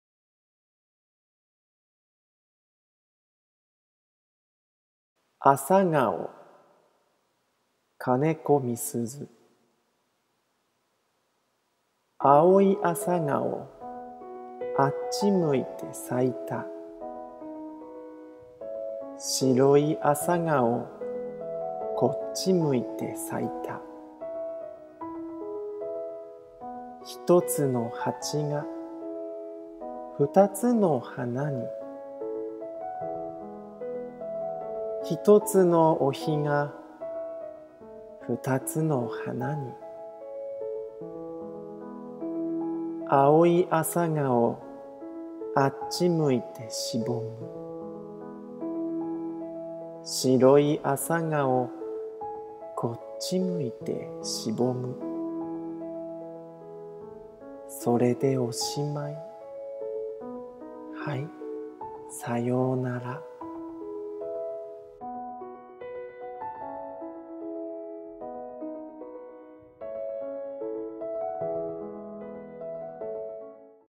Japanese Poetry Reading " a morning glory (あさがお)" by Misuzu Kaneko 金子みすゞ
Easy listen to Relax.
Japanese Poetry Reading.

 " a morning glory (あさがお)"
by Misuzu Kaneko (金子みすゞ), a famous female poet, Japan.